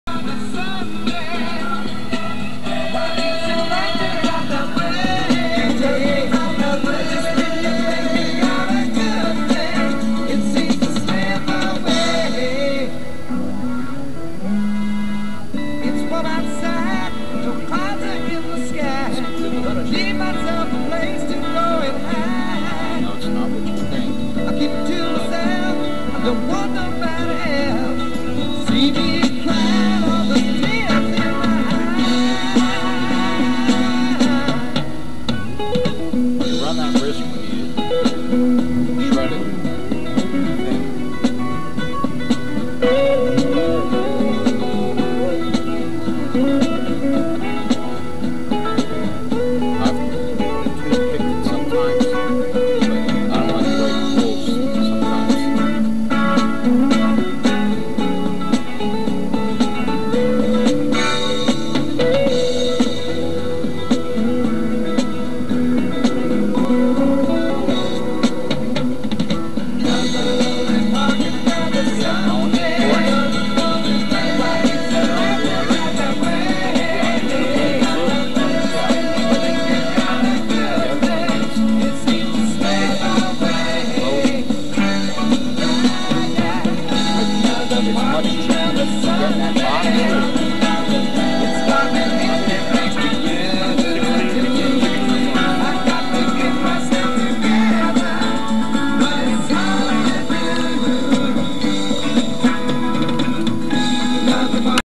special sauce
brad kay applying the special secret sauce to the chicken. this is at the 2013 fire ant festival in ashburn ga.